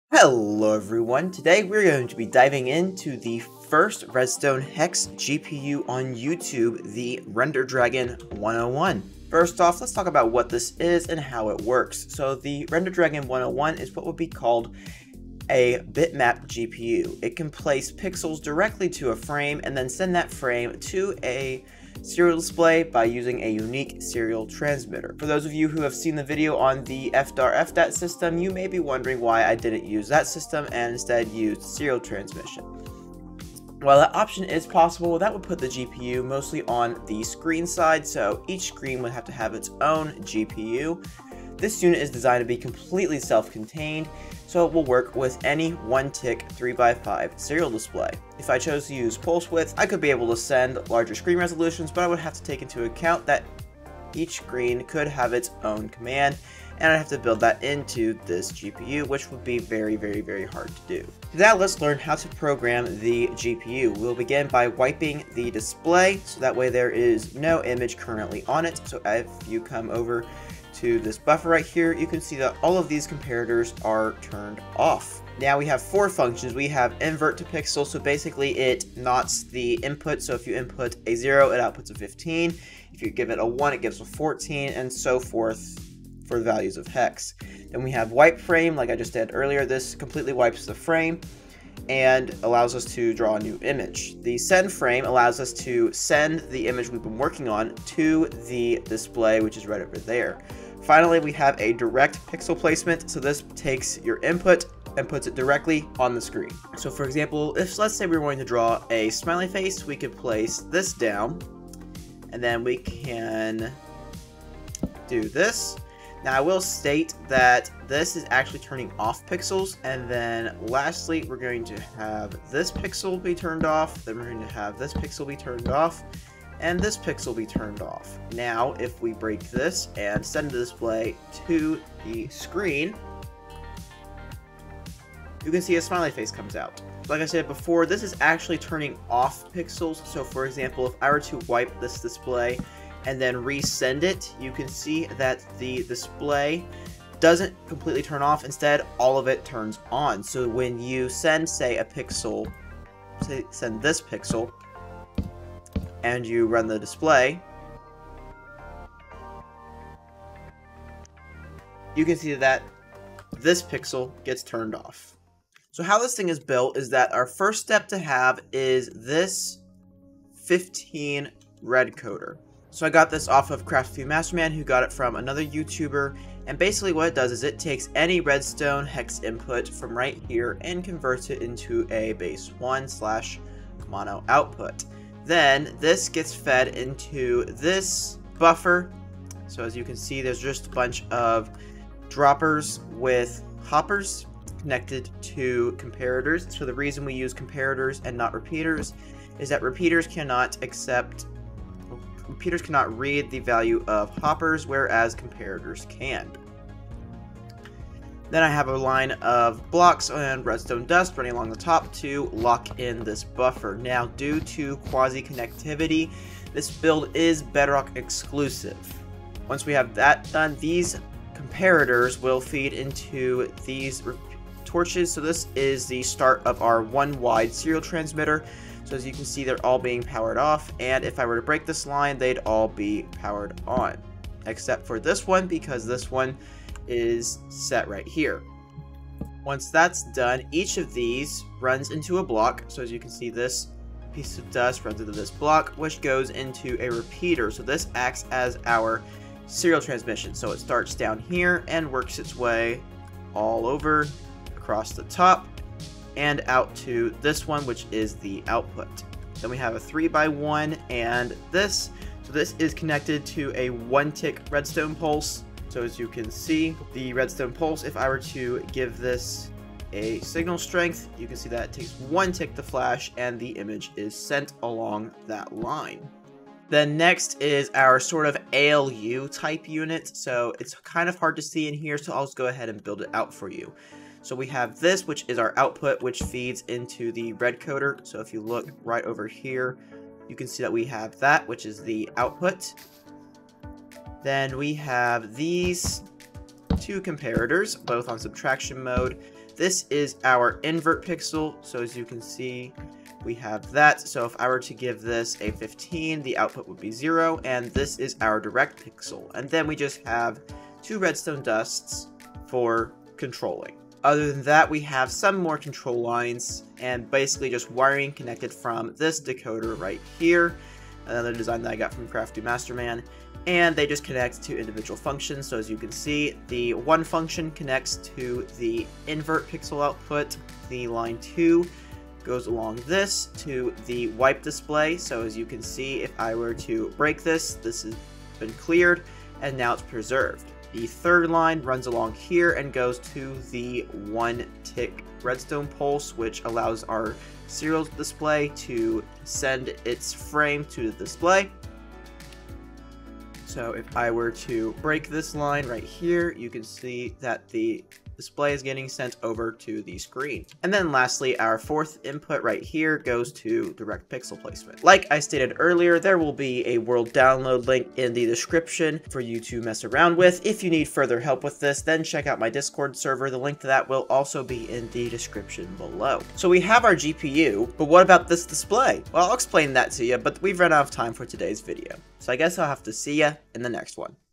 RenderDragon 101 - Redstone Hex GPU
This is the RenderDragon 101, the first redstone hex GPU on YouTube!

Timestamps:
0:00 - Introduction
0:10 - What is This?
1:01 - Programming
2:45 - How it's Built
6:05 - World Download